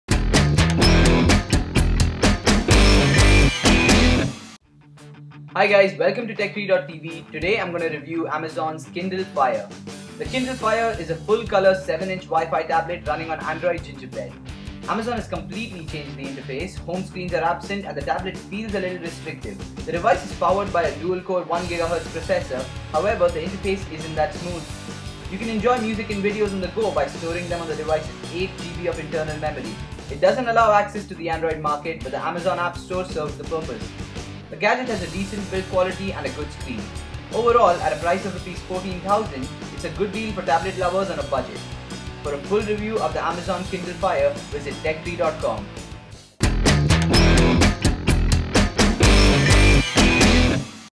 TechTree.tv: Kindle Fire Video Review
Is it an ebook reader, or is it a tablet? In this edition of TechTree.tv, we check out the Amazon Kindle Fire. Although its features look promising, a partial implementation of Android Gingerbread is a dampner. Let us find out how the Amazon's first colour tablet fares.